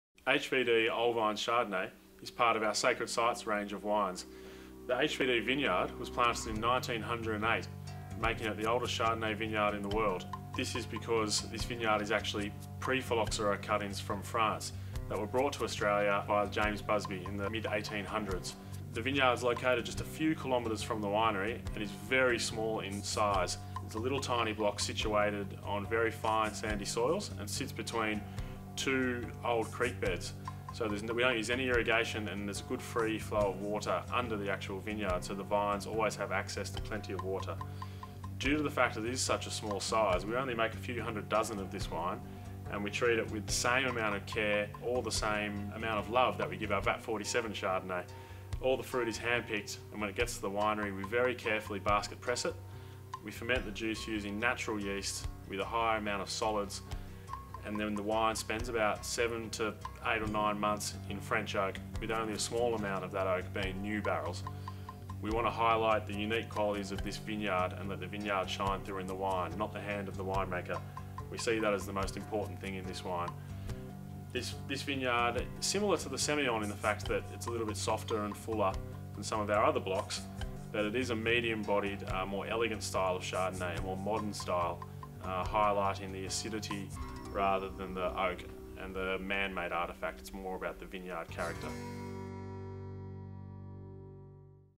Tyrrell's Wines HVD 'Old Vines' Chardonnay
Chris Tyrrell discusses the history and style of Tyrrell's HVD 'Old Vines' Chardonnay, which comes from vines planted in 1908.

Grapes for this wine are sourced from the HVD (Hunter Valley
Distillery) vineyard, situated between two dry creek beds. The grapes were taken from the young block with cuttings from the old vines.